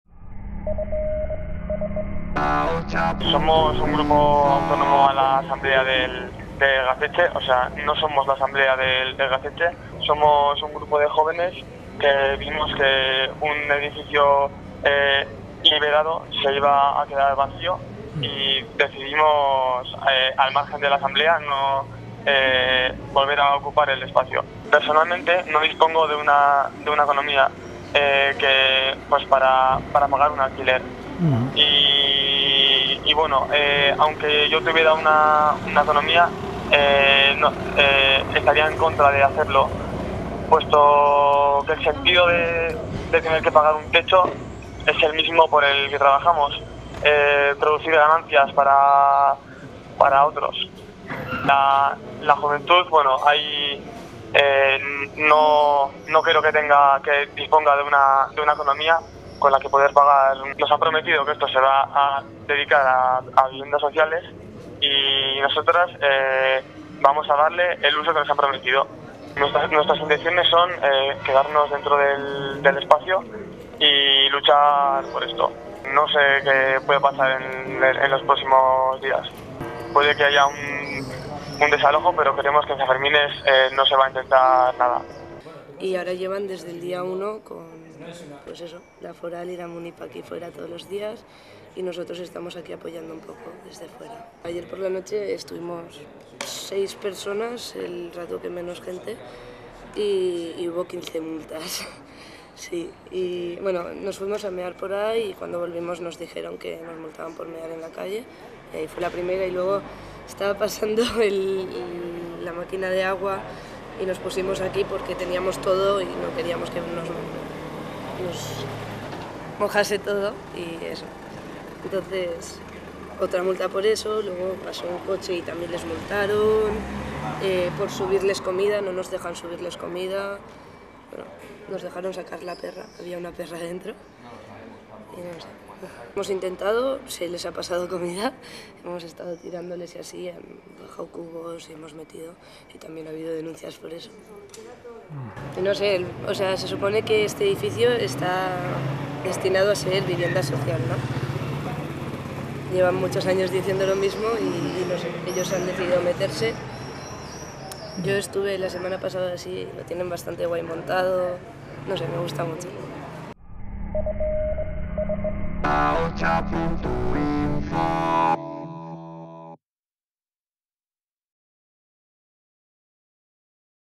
"Este edificio está destinado a viviendas sociales y nosotrxs le daremos ese uso"
Los okupas de Compañía 3 han declarado a Ahotsa.info que no tienen intención de abandonar el inmueble pese a la presión policial. Un edificio que se encontraba vacío y que estaba destinado a vivienda social. Ahora, un grupo de jóvenes sin recursos económicos para pagar un alquiler, lo han ocupado.